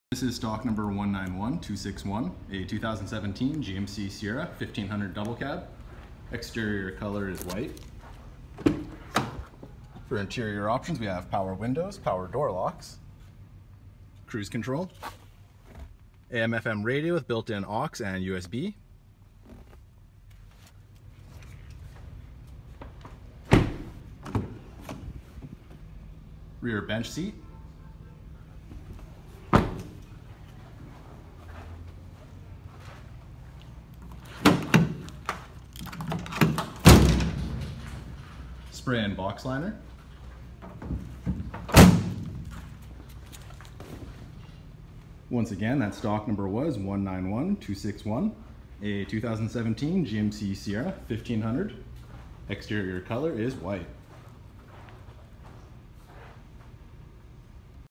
191261
Are you tired of all the bells and whistles and just want a plain ol’ work truck? Well, here it is! This beauty has cloth interior with seating for 6. It’s a one owner, clean CARFAX truck. It also has air conditioning, cruise control, and an aftermarket remote starter with power lock/unlock. Great for the farm, first time truck buyer, and anyone who’s on a budget. Come by for a test drive today! Comes complete with a lube, oil, filter change and an ANVIC approved inspection. (Uploaded by DataDriver).